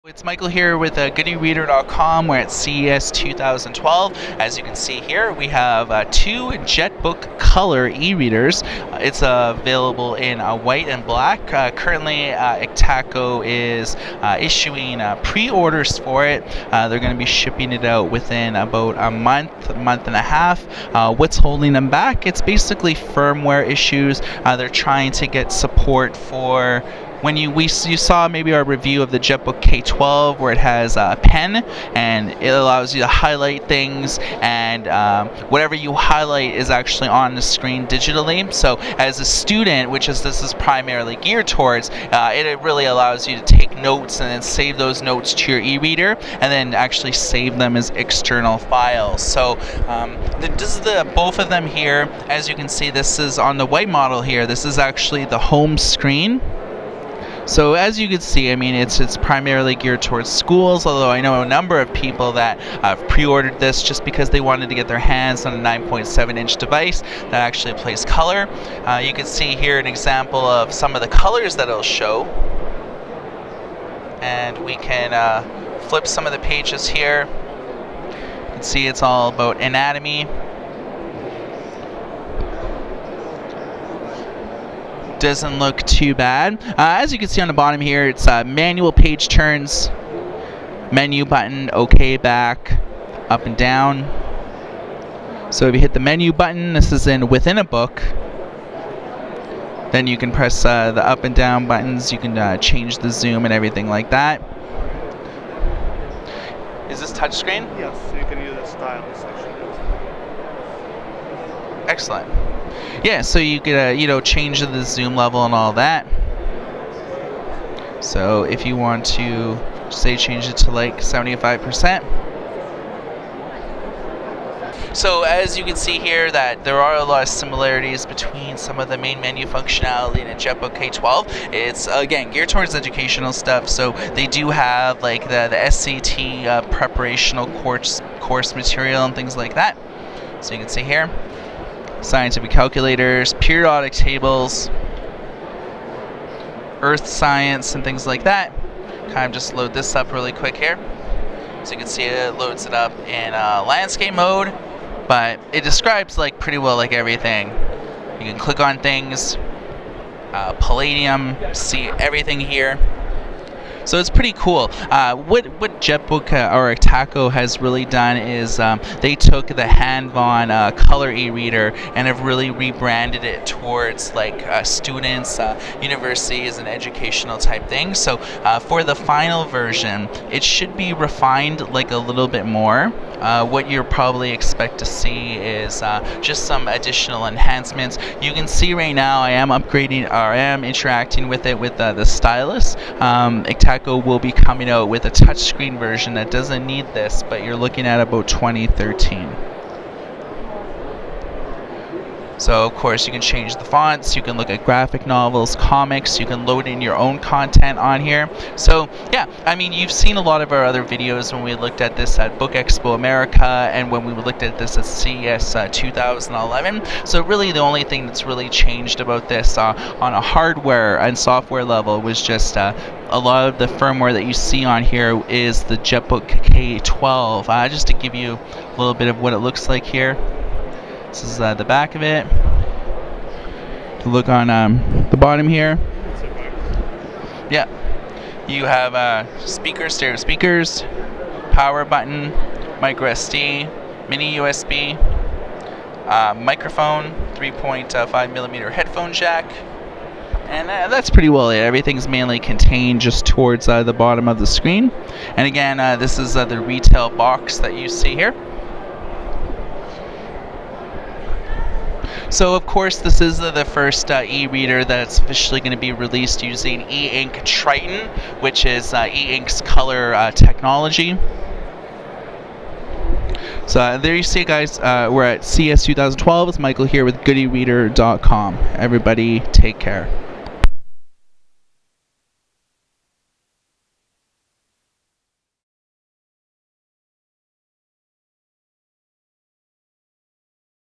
Ectaco Jetbook Color eReader at CES 2012
We give you the first look at the final firmware you will see in the first colour e-reader using e-ink Triton. Check it out for a full overview of all the main apps and programs.